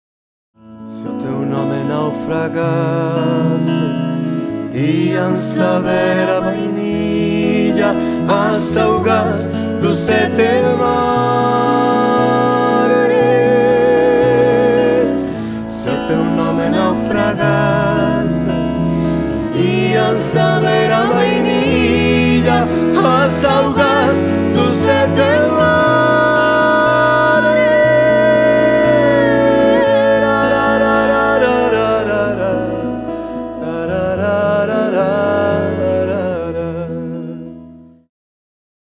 Concertos aCadaCanto
aCadaCanto, próximas datas de concertos:

- 8 de xuño ás 20.30h no Teatro Rosalía, A Coruña.
-13 de xuño ás 21h no Teatro Principal, Compostela.